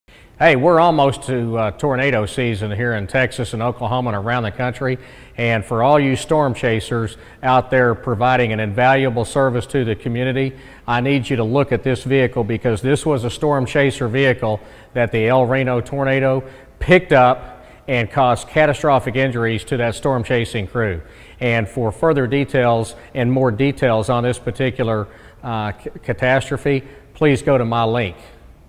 Tornado Chasers Safety Advisory
Todd Tracy represents a storm chaser who was seriously injured in the Weather Channel's Tornado Hunter 2013 SUV when it was picked up by the El Reno, Oklahoma tornado.

Tracy and his team of experts want you to understand that storm chasing in a production model vehicle not equipped with special safety protection to protect occupants can have a  deadly result.